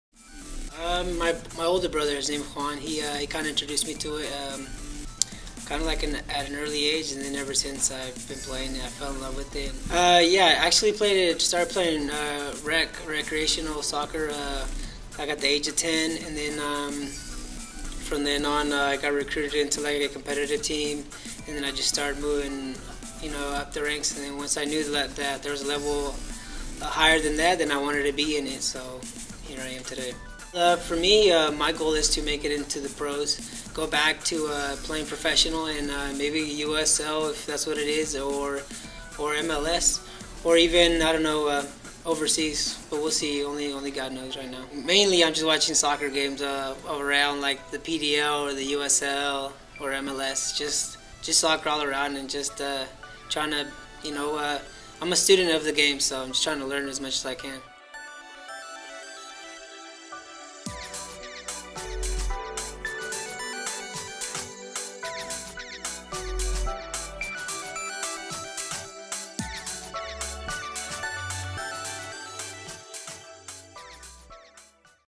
Luis Martinez | Desert Diamond Casinos
Let's meet FC Tucson's razor sharp midfielder, Luis Martinez. Read his full player profile, presented by Desert Diamond Casinos